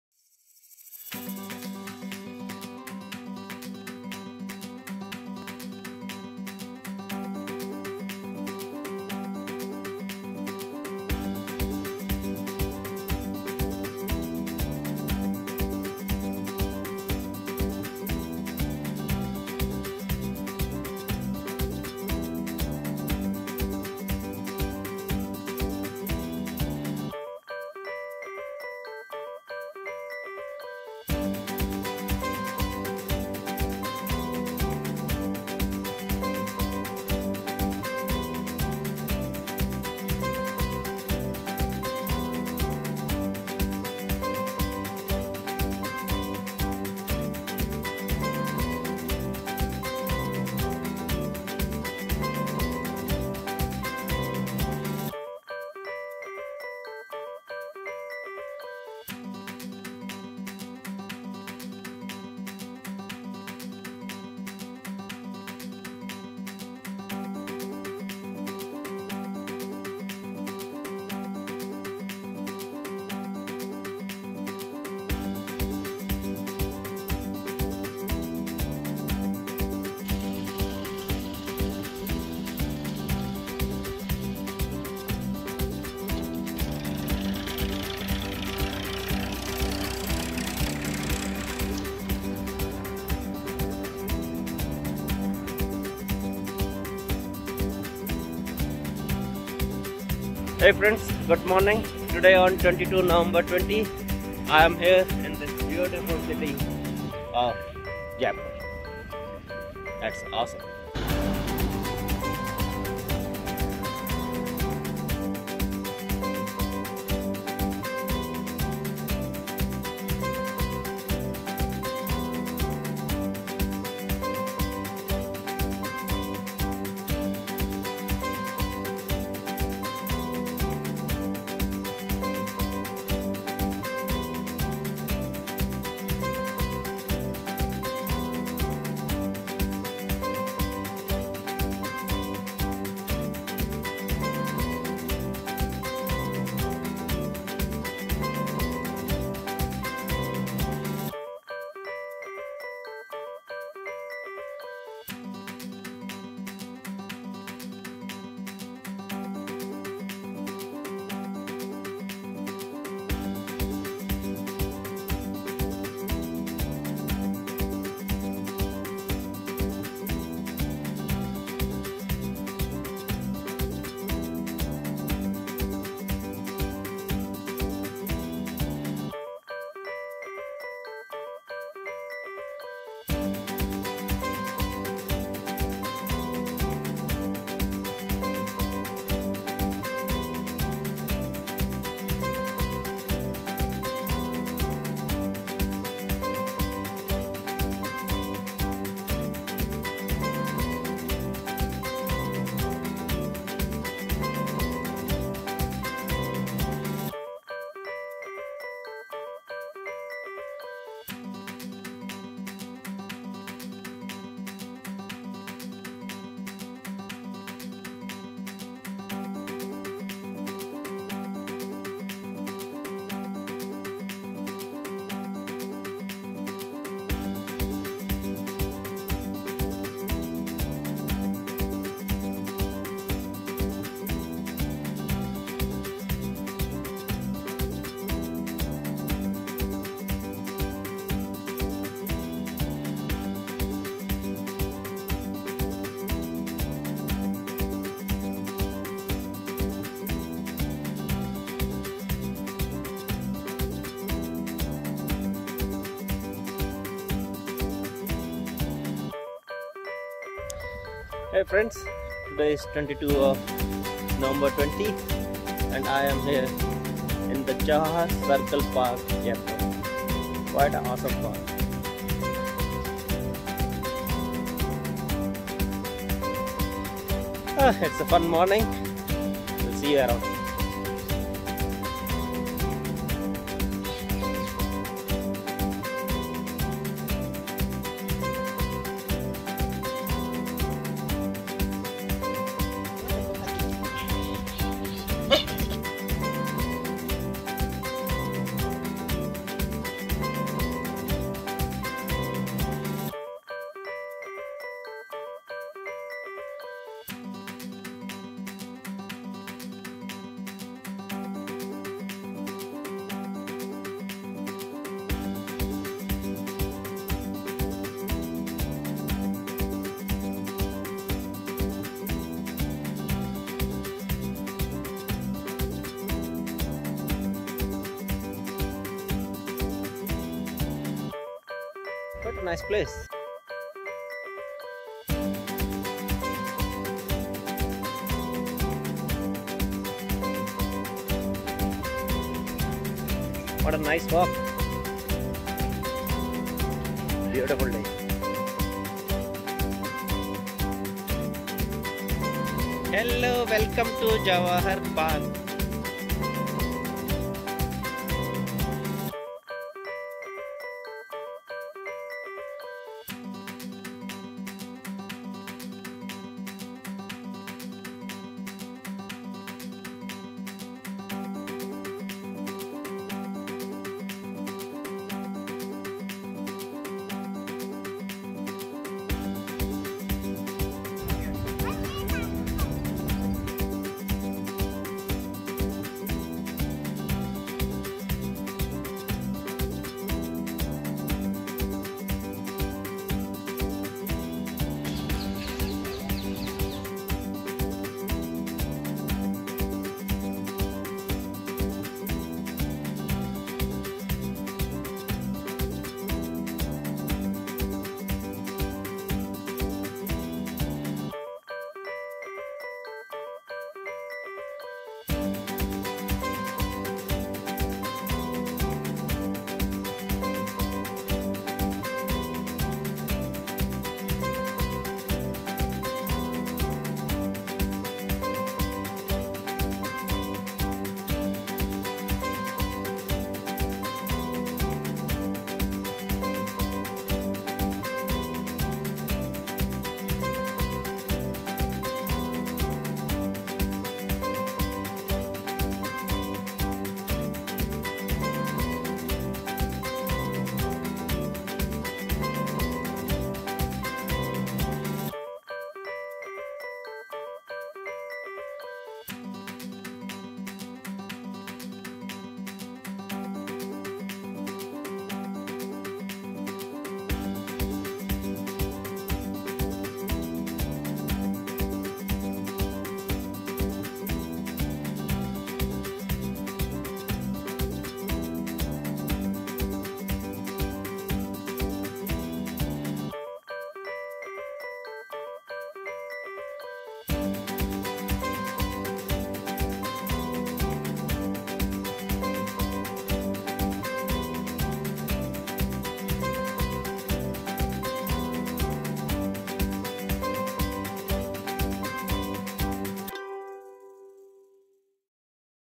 Day1-2. meeting old dudes & morning at.. Jawahar circle park ..Jaipur tales!
Jaipur visit 2020.